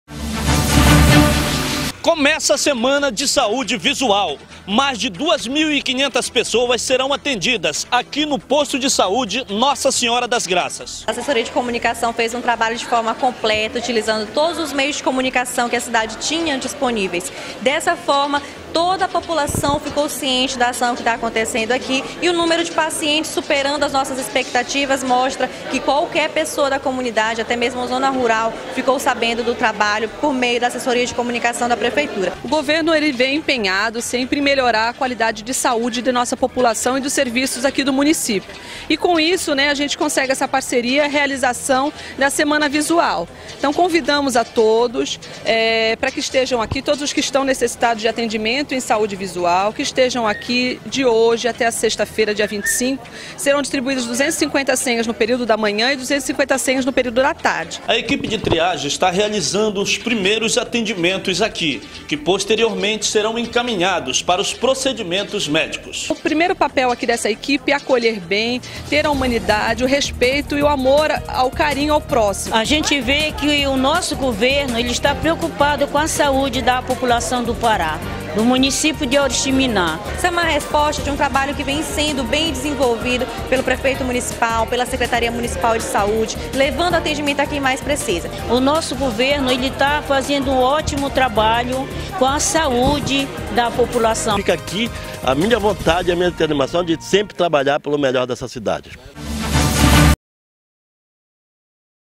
Semana Visual em Oriximiná
● Em Oriximiná o Governo Ludugero Tavares se preocupa com a saúde da população, trabalha forte na prevenção de doenças, a Semana da Saúde Visual que iniciou na segunda, tem como objetivo atender mais de duas mil e quinhentos pessoas – Assim que deve ser!